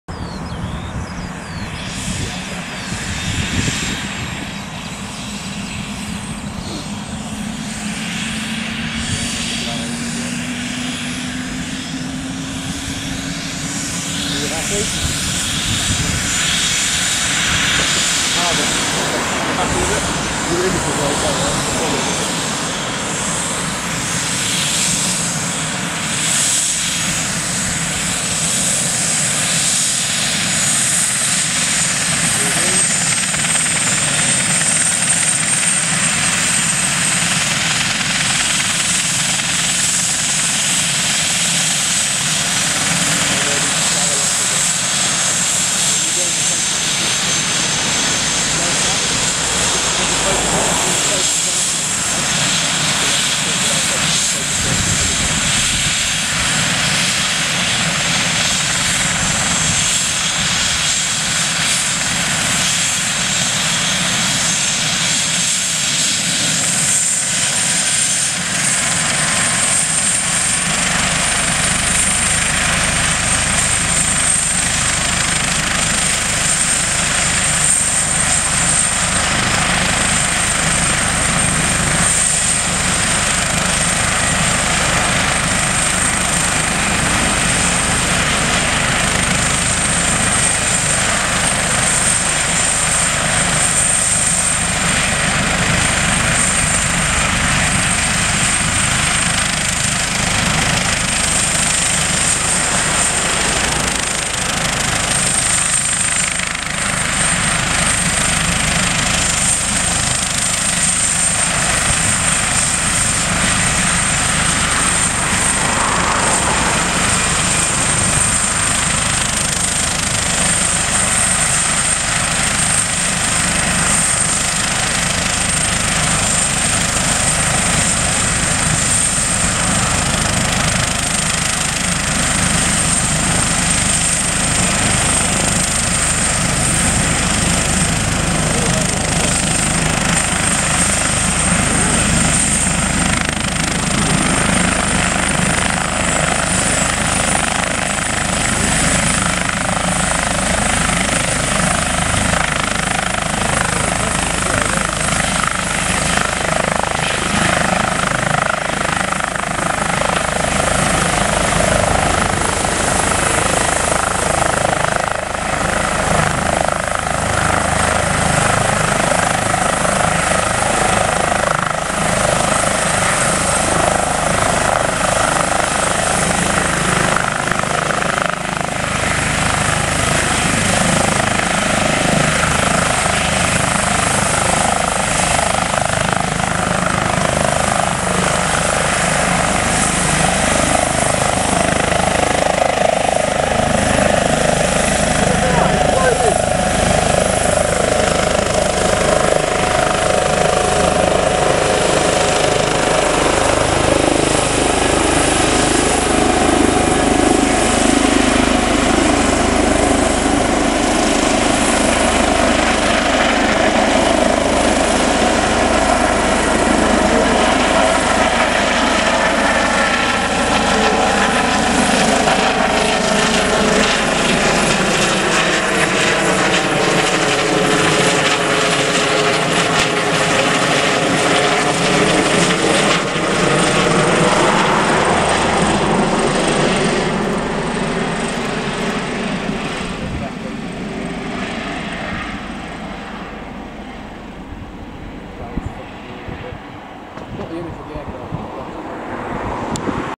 *HELIMED-88* ~ East Anglian Air Ambulance taking off from Addenbrookes Hospital.
East Anglian Air Ambulance, Callsign HELIMED-88 based out of the Cambridge Base. Seen here taking off from the pad at Addenbrookes Hospital after dropping off a patient.